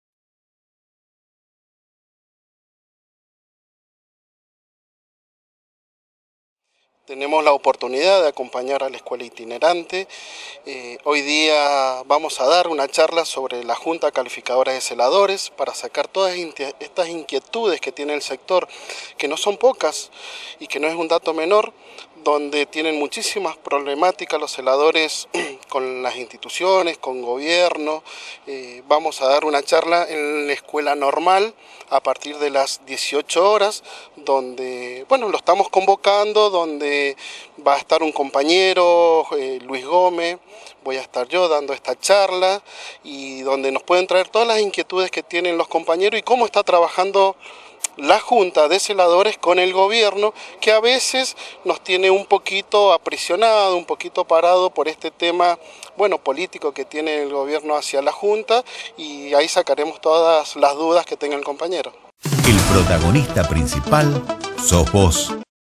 MAYOR PROBLEMÁTICA EN EDUCACIÓN
A los conflictos gremiales en el sector de los trabajadores de la educación en nuestra provincia, se suman los celadores escolares con charlas esclarecedoras como una forma original y pacífica de protesta.